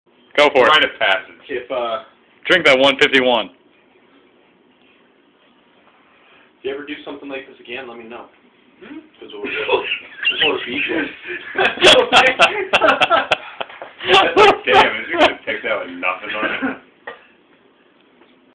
Louis tries Bacardi 151
Joe had less than a shot left of Bacardi 151, and Louis wanted to try it.

Ive tried that stuff, my god, it burns!

NOTE: We are all responsible drinkers, and over the age of 21.